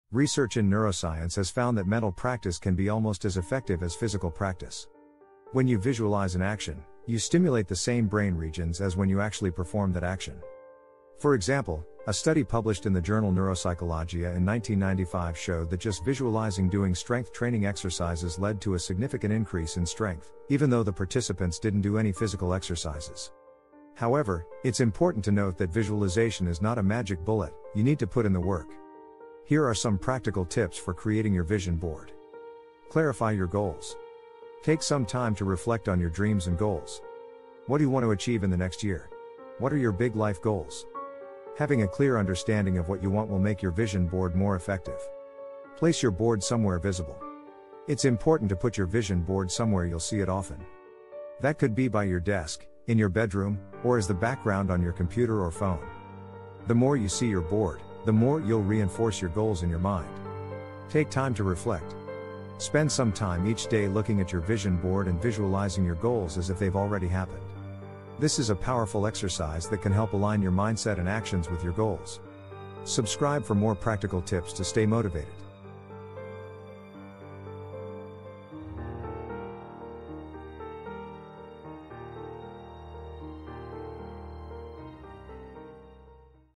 Manifest Your Dreams: How to Create an Effective Vision Board
🔑 Key Takeaways from this Video:
The Power of Visualization: Visualization is a potent tool that stimulates the same brain regions as when you actually perform an action. This stimulation strengthens neural pathways associated with the envisioned action, leading to improved performance.

Vision Boards as a Tool for Goal Setting: A vision board is an effective way to visually represent your goals and aspirations. It acts as a constant reminder of what you want to achieve, fostering a daily habit of visualization.

Creating an Effective Vision Board: Clarifying your goals, placing your vision board somewhere visible, and spending time each day to reflect and visualize your goals as already achieved are critical steps for creating a successful vision board.

Vision Boards and Motivation: Vision boards can help keep you motivated by providing a constant visual reminder of your goals, fostering positive thinking, and aligning your mindset and actions with your aspirations.